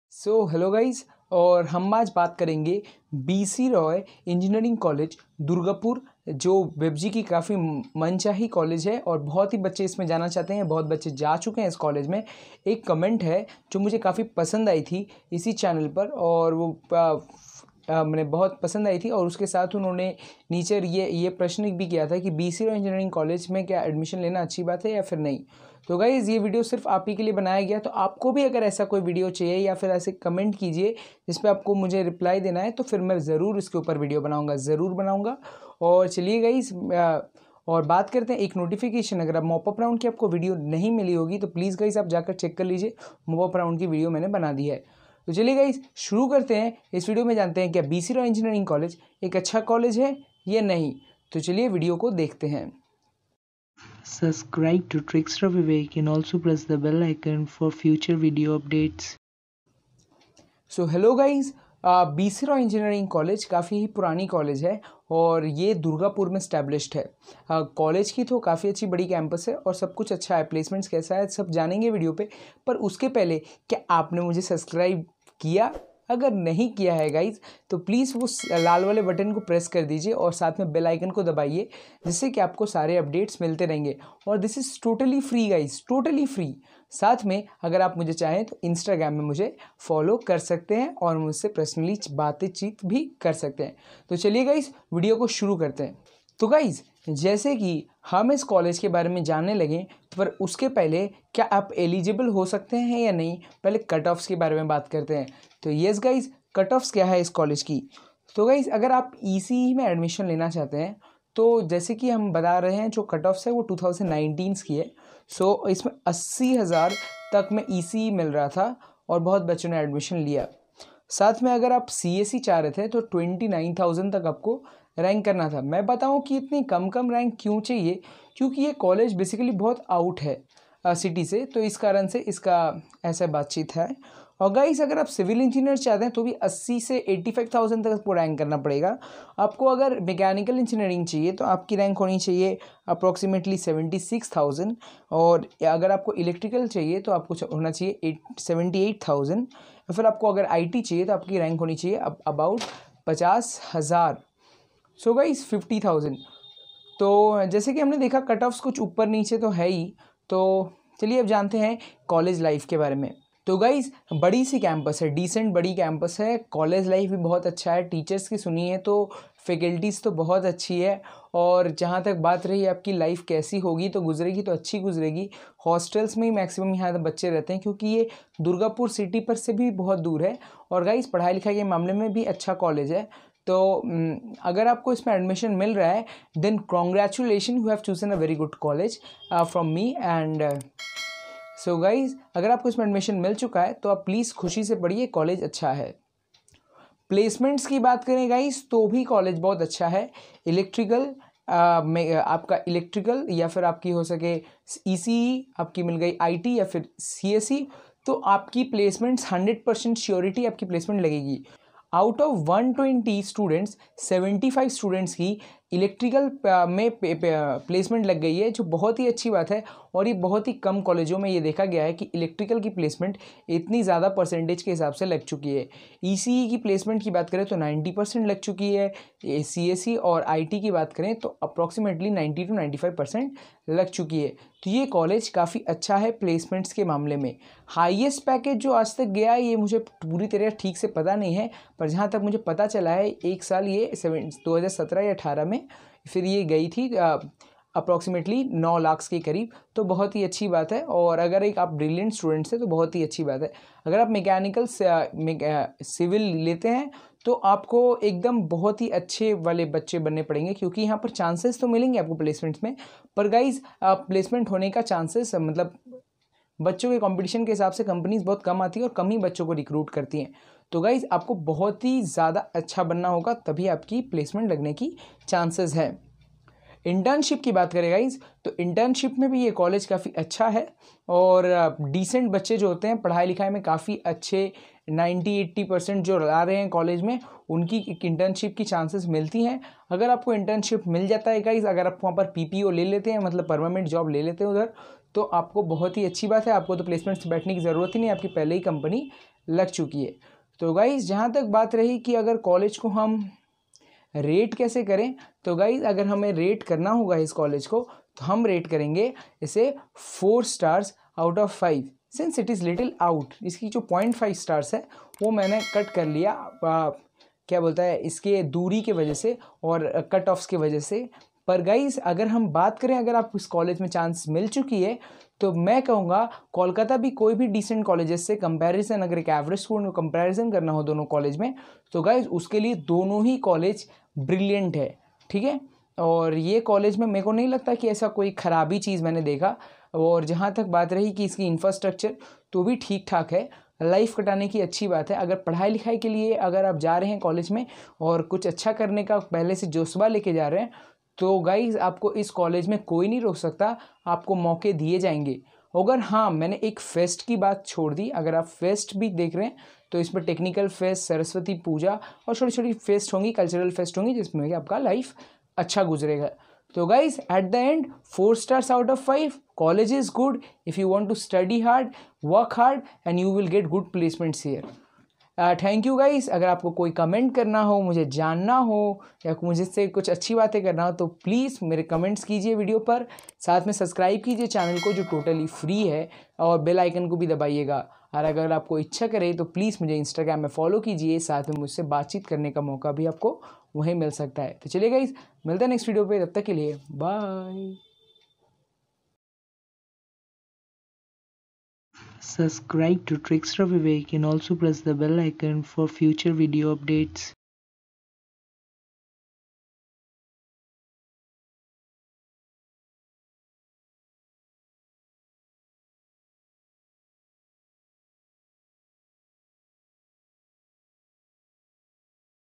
Dr B C Roy Engineering College - Durgapur (BCRE) ||Cutoff|| Placements || Low Wbjee rank
Excellent 👌 Wbjee College and accepts Low rank BREC - Dr B C Roy Engineering College - Durgapur.
How are the Placements , Cut off and The faculty we discuss .
Must Follow to get daily updates on Time .